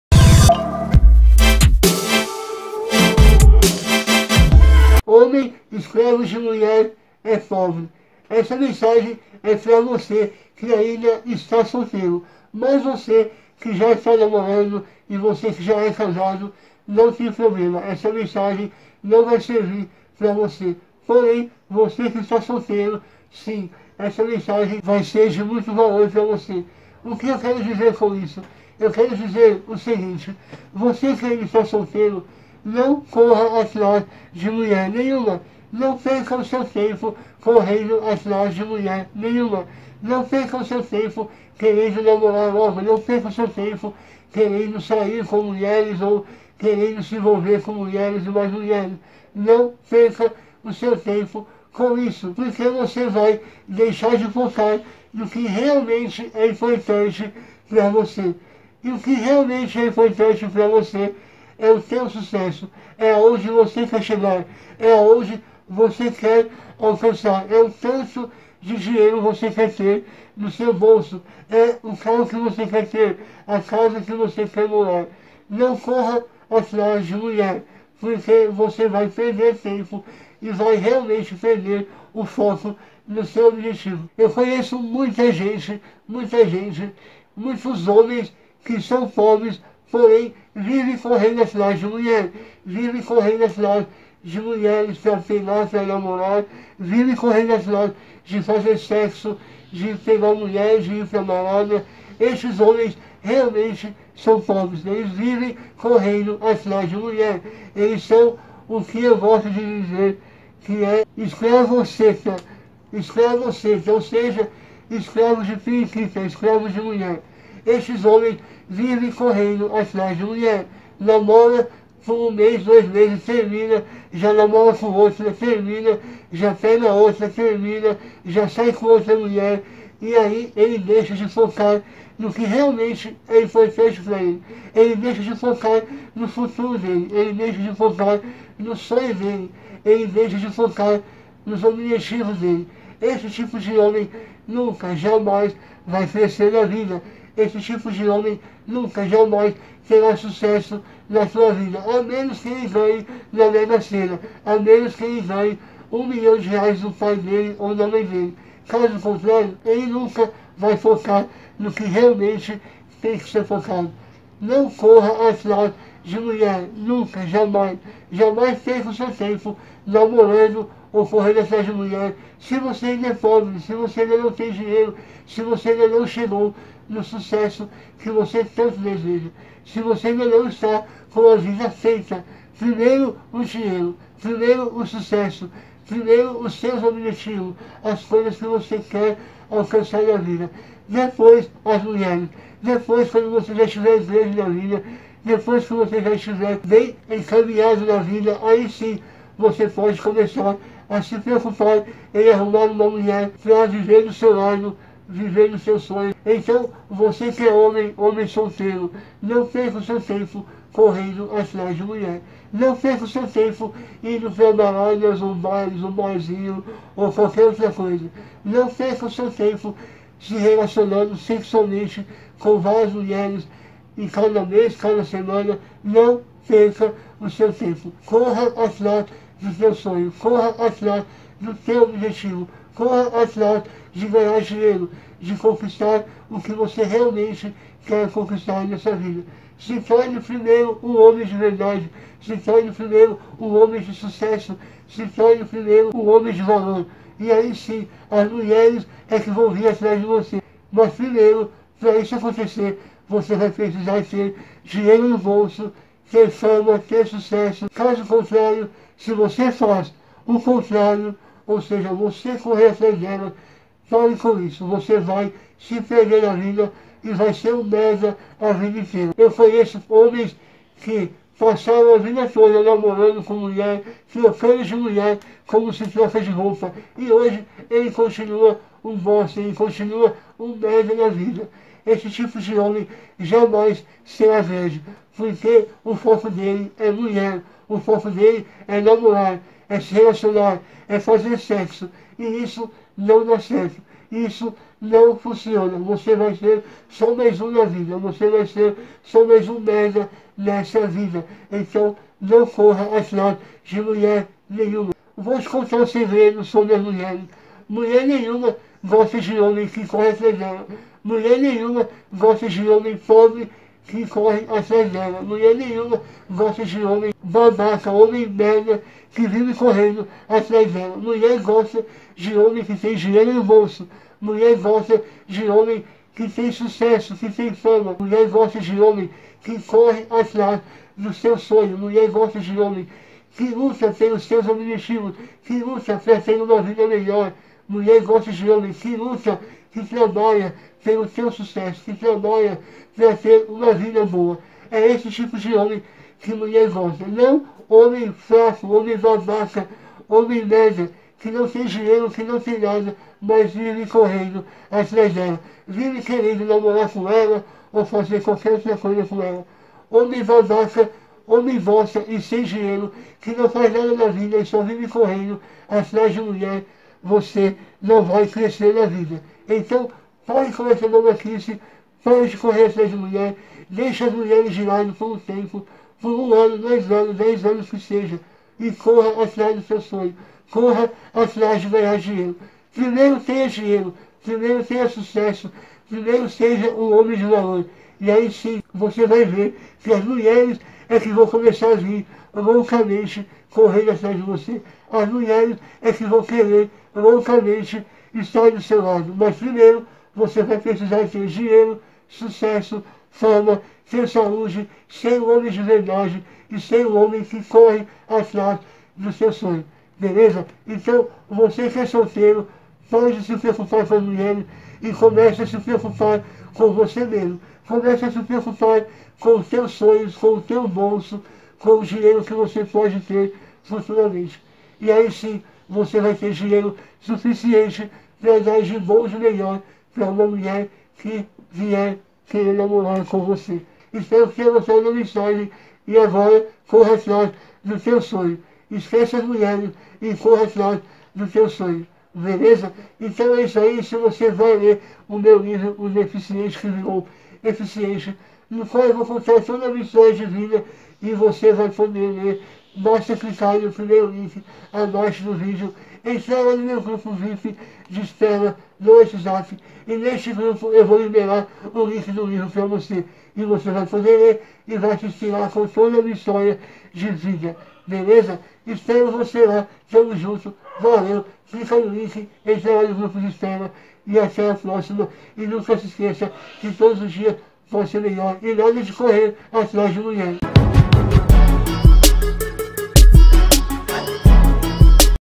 HOMEM NÃO CORRA ATRÁS DE MULHER SE VOCÊ AINDA NÃO TEM SUCESSO NA VIDA | Fabio Lemos
homens não corram atrás de mulher antes da hora, aprendam correr atrás de seus sonhos primeiros e conquistarem uma vida maravilhosa pra depois pensar e ter mulher na sua vida